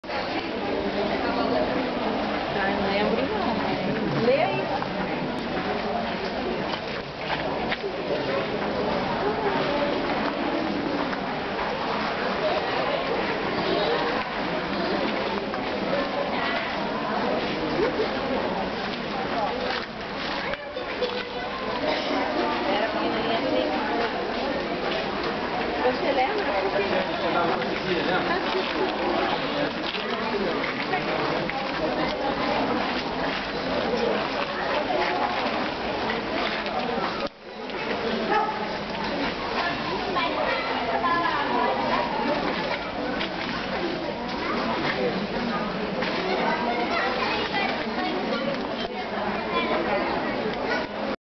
orquideas no jardim botanico parte 1
Orquideas em exposição no Jardim Botanico, visita em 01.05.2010